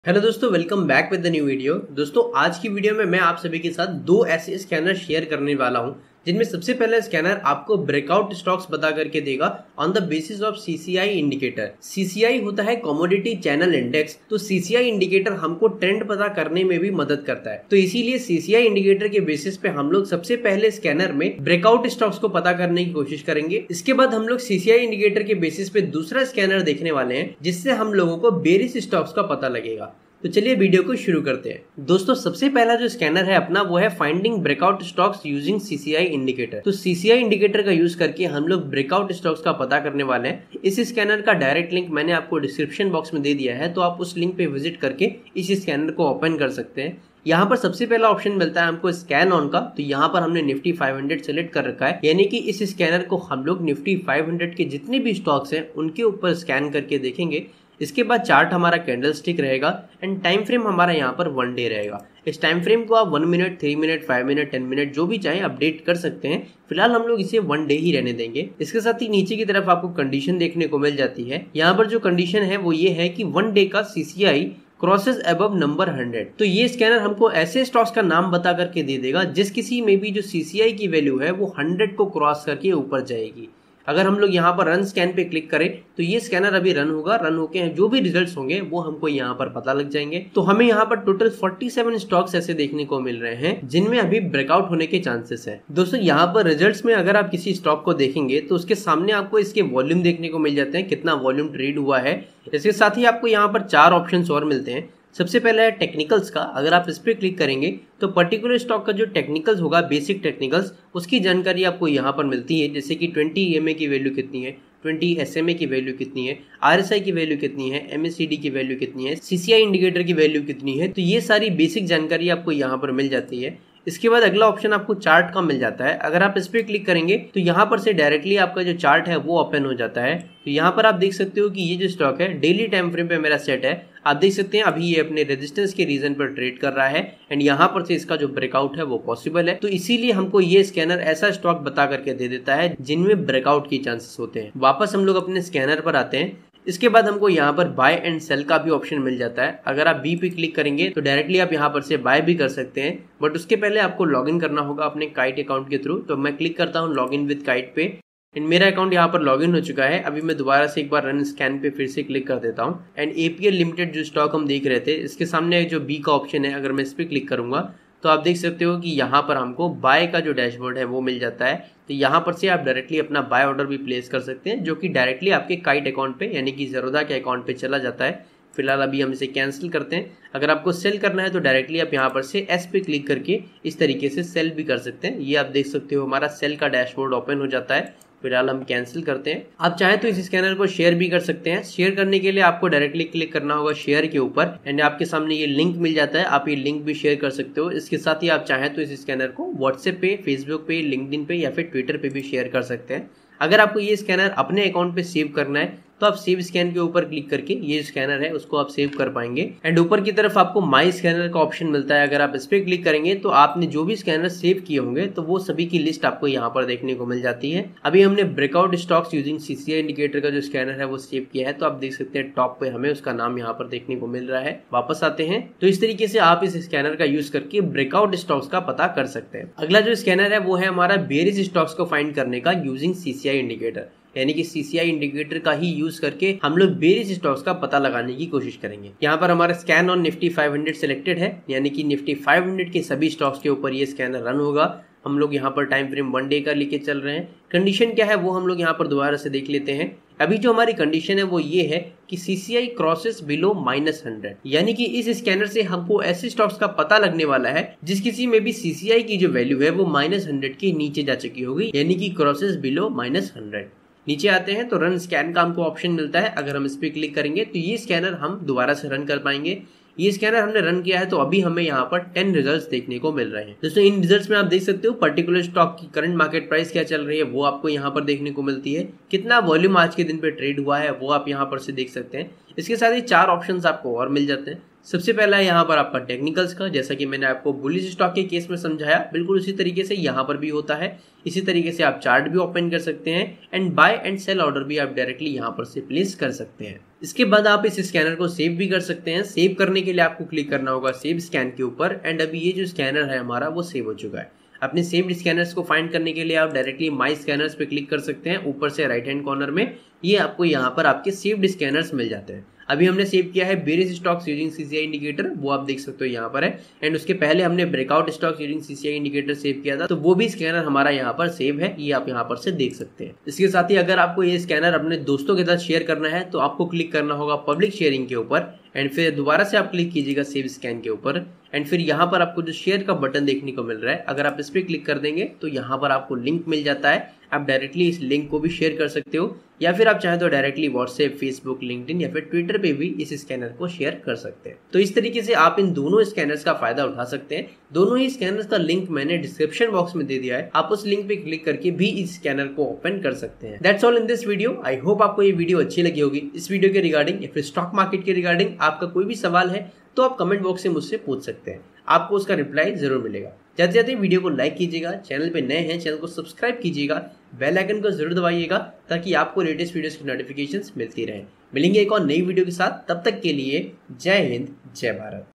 Breakout stocks using CCI Indicator || Streak Scanner
In this video, you will get to know about finding breakout stocks using cci indicator and streak scanner.